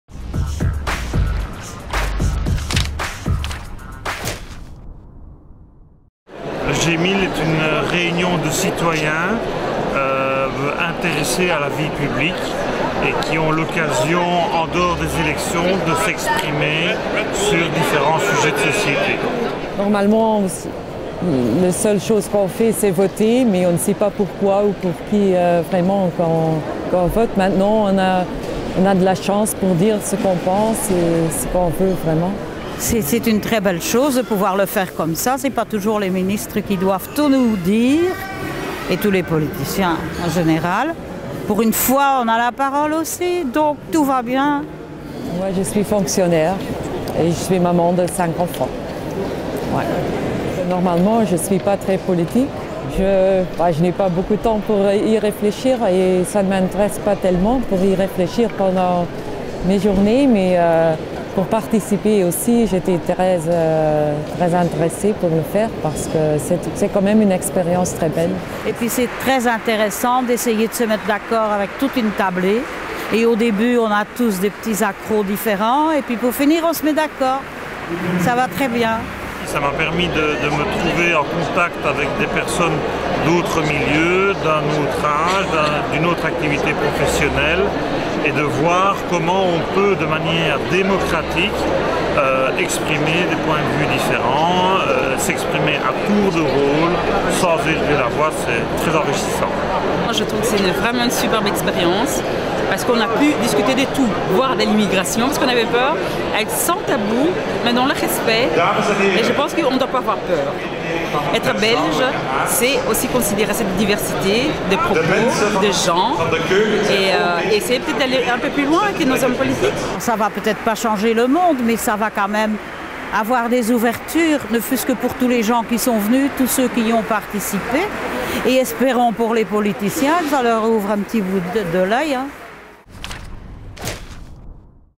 G1000 (CTVS1/E11)
Léthargie proscrite et effets dynamisants garantis :

Le 11 novembre dernier, à Tour et Taxis, le G1000 réunissait 750 personnes tirées au sort pour débattre des sujets les plus brûlants du mois. Une expérience nouvelle de démocratie participative que Corsaires TV n'a pas hésité à suivre.